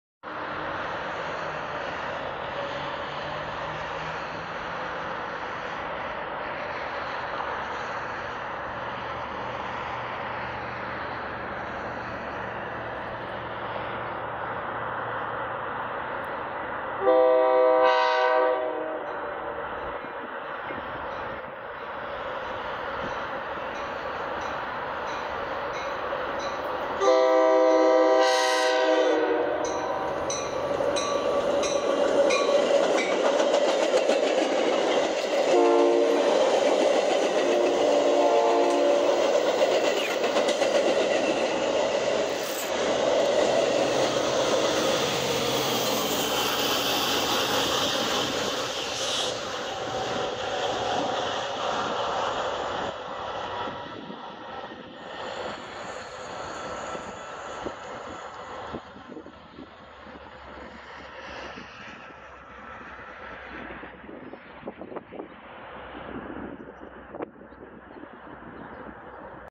Metra cab-car 8525 east at Elgin, Illinois with SD70MACH 502 on November 1, 2024
Metra cab-car 8525 leads a Chicago bound morning rush train through Elgin, Illinois near McLean Blvd. with SD70MACH 502 for power on November 1, 2024.